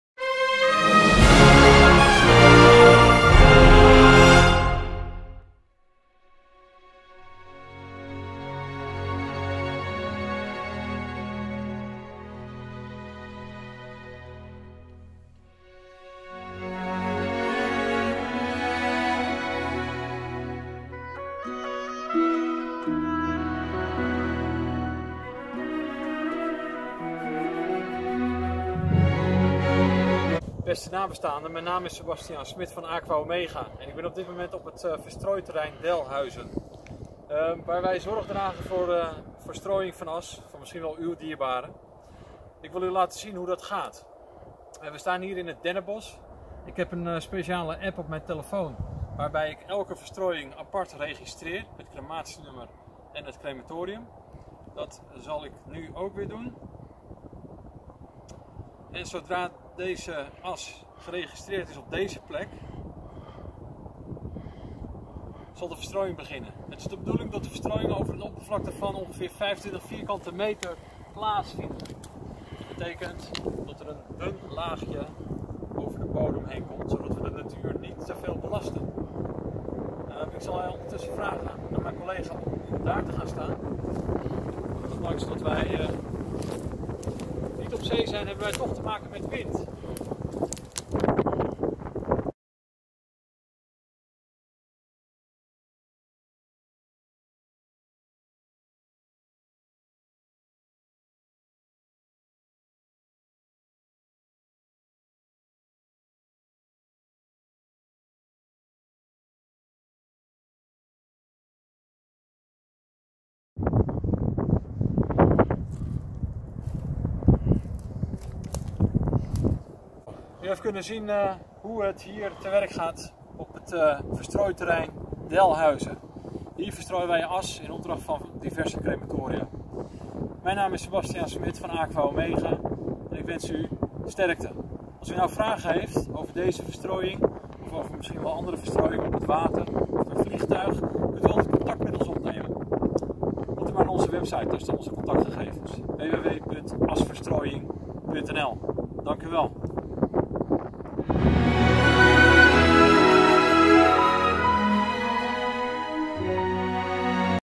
Asverstrooiing op verstrooiterrein Delhuyzen
Asverstrooiing in de natuur op het verstrooiterrein Delhuyzen

Algemene informatie
Het natuurgebied is een mooie rustige plaats, waar waardige asverstrooiing verzekerd is. Het gebied bestaat uit heidevelden, dennenbossen en loofbomen als eiken, berken en beuken. Hier en daar staan er bankjes waar u kunt zitten om van de natuur en rust te genieten.

Ecologisch evenwicht
Delhuyzen wordt met zorg onderhouden en de ecologische balans is belangrijk. Om het natuurlijke karakter van het terrein te waarborgen is het niet toegestaan om zelf struiken, bomen, gedenktekens of (brandende) kaarsen te plaatsen.

Openingstijden
Het terrein is dagelijks geopend van zonsopkomst tot zonsondergang. Uit respect voor de mensen van wie de as op Delhuyzen is verstrooid, is het wenselijk dat u op de aangeduide paden blijft. Honden zijn, mits aangelijnd, welkom.

Route
Nationaal Verstrooiterrein Delhuyzen ligt aan de Apeldoornseweg in Arnhem. Het terrein is het makkelijkst met eigen vervoer bereikbaar en ligt zo'n 3 minuten vanaf de snelweg. Volg de Apeldoornseweg richting Vliegveld Terlet. De parkeerplaats ligt ter hoogte van hectometerpaal 5,3. Er is parkeergelegenheid bij het terrein, maar houd op zondagen rekening met drukte door recreatieve bezoekers. De dichtstbijzijnde bushalte is bij het golfterrein; van daaraf is het nog 20 minuten lopen.